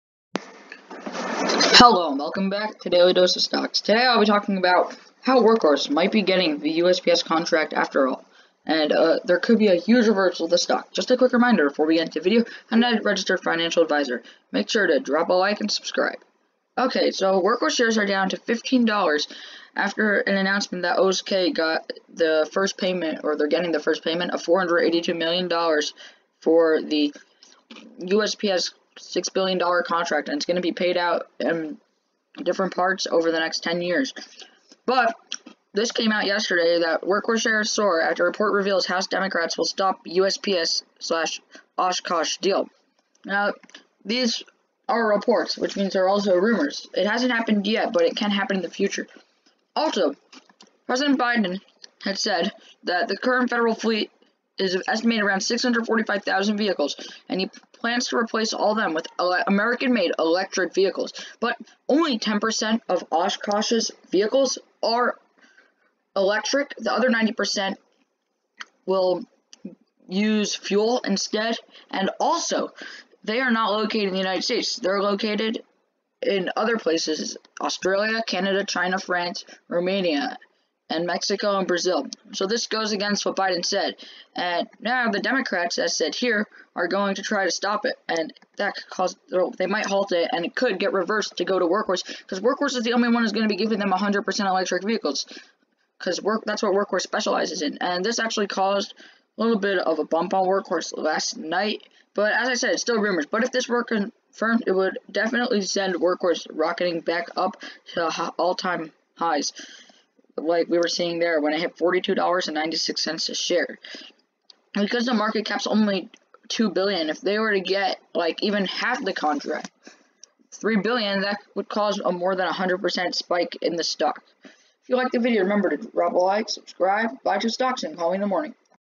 WKHS Soars after a Rumor that House Democrats will stop OSK from receiving the contract
Hello and welcome back to daily dose of stocks. today I talk about a new rumor that is spreading about the usps contract. if this is true wkhs can explode back up to 40 dollars a share.